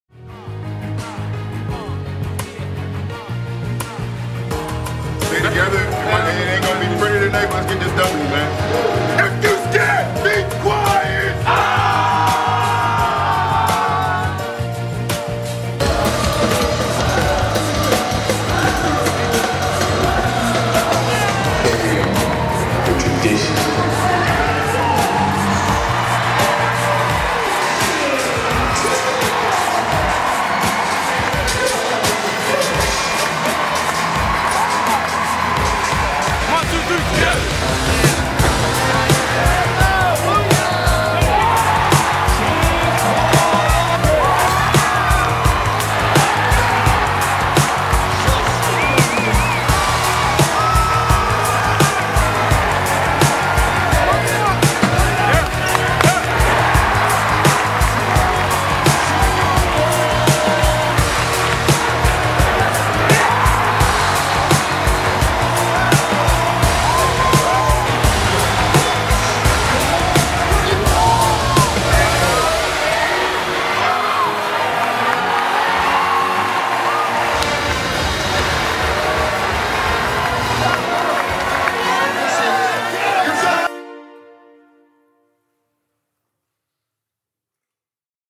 Dominating Victory over Lasalle
Ram Nation brought it again and helped VCU blow out La Salle